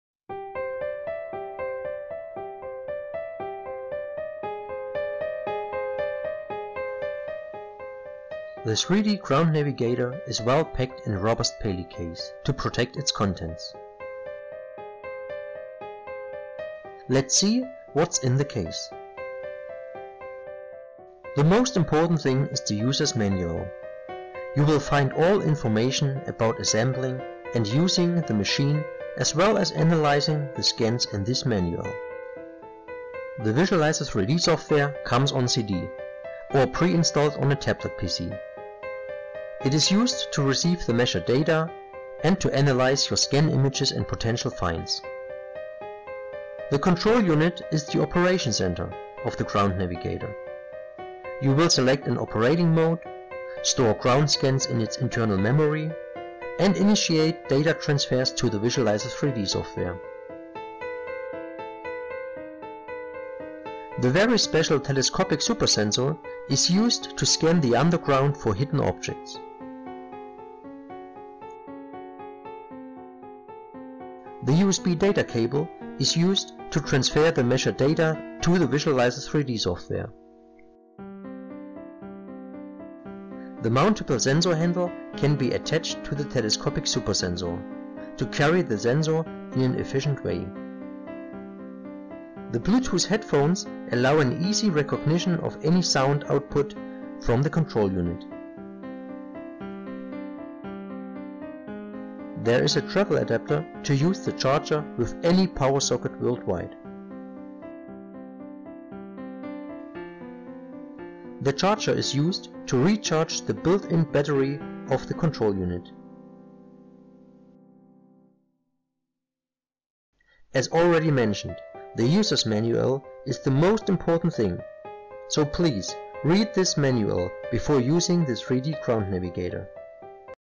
3D Ground Navigator Gold Detector - part 1
The Latest 3d Imaging Technology

3d Ground Navigator Is A Modern 3d Ground Scanner And Metal Detector That Has Been Manufactured Exclusively By Okm Metal Detectors From Germany.
The Detector Has Been Developed According To Our Very Specific Customer Demands.
Modern Geophysical Detection Instrument To Locate Buried Artifacts, Lost Treasures, Subsurface Anomalies, Underground Utilities As Well As Other Hidden Structures. It Provides Nondestructive Examination Methods To Render Graphical Images Of Subterranean Targets Before Excavation.

This Is A New Metal Detector – The 3D Ground Navigator. We German Group Established A Co-Operation With The German Metal Detector Manufacturer OKM And Ordered An Exclusive Ground Scanner, That Has Been Modeled According To Our Demands And The Needs Of Our Customers.